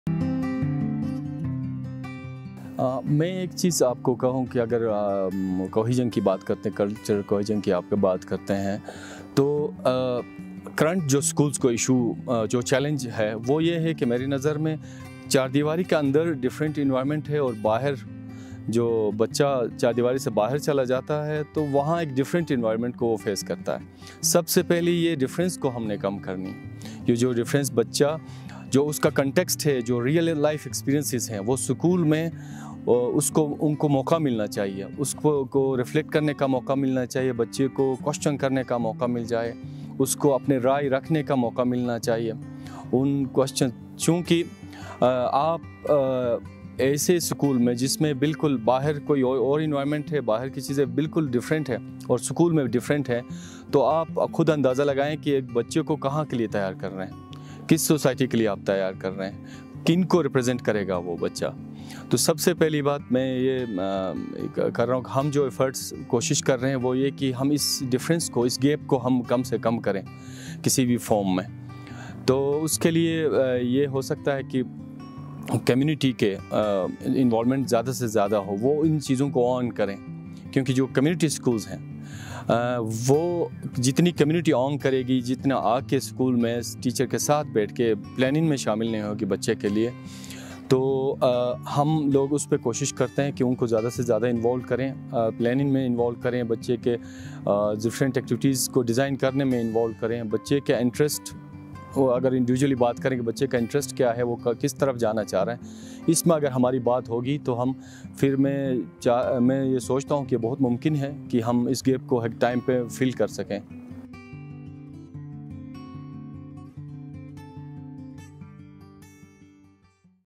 Dichotomy | Effective learning environment for cultural cohesion
Mr. Shoukat Shahabuddin discusses possibilities to create an effective learning environment for children in terms of cultural cohesion.